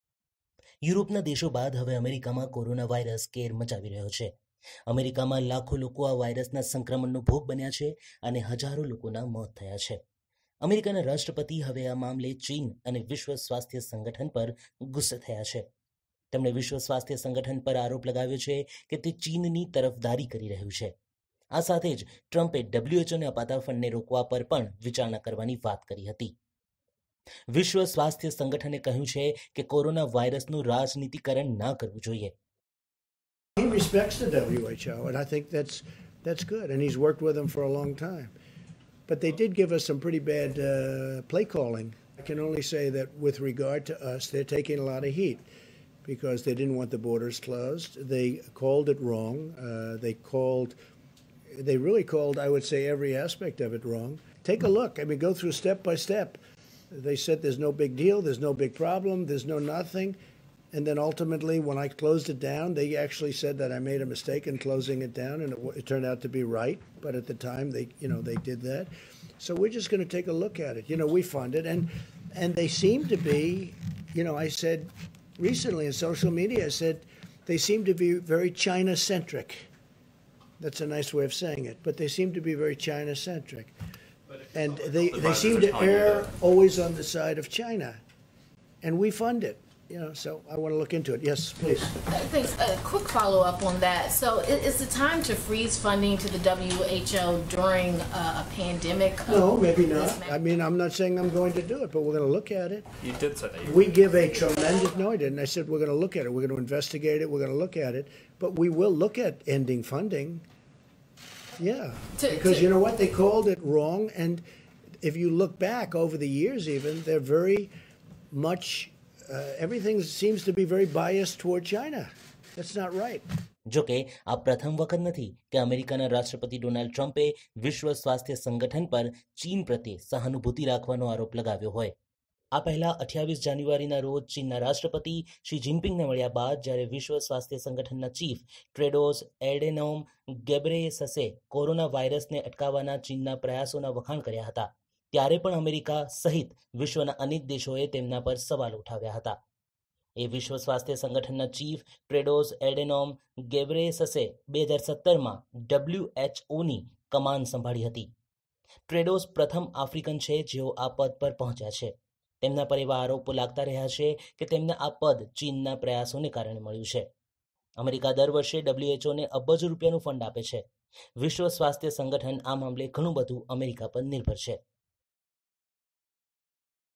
Donald Trump હવે WHO અને ચીન પર કેમ ભડક્યા, શું છે મામલો?
યુરોપના દેશો બાદ હવે અમેરિકામાં કોરોના વાઇરસ કેર મચાવી રહ્યો છે. અમેરિકામાં લાખો લોકો આ વાઇરસના સંક્રમણનો ભોગ બન્યા છે અને હજારો લોકોનાં મોત થયાં છે.
અમેરિકાના રાષ્ટ્રપતિ હવે આ મામલે ચીન અને વિશ્વ સ્વાસ્થ્ય સંગઠન પર ગુસ્સે થયા છે.  તેમણે વિશ્વ સ્વાસ્થ્ય સંગઠન પર આરોપ લગાવ્યો છે કે તે ચીનની તરફદારી કરી રહ્યું છે.
ટ્રમ્પે તો ત્યાં સુધી કહ્યું કે WHOએ કોરોના વાઇરસ મામલે ઘણી ખોટી જાણકારી આપી છે.  આ સાથે જ ટ્રમ્પે WHO ને અપાતા ફંડને રોકવા પર પણ વિચારણા કરવાની વાત કરી હતી..

તમે અમારી સાથે જોડાઈ શકો છો :

JioChat Channel : BBC Gujarati
Helo : BBC News ગુજરાતી
ShareChat : bbcnewsgujarati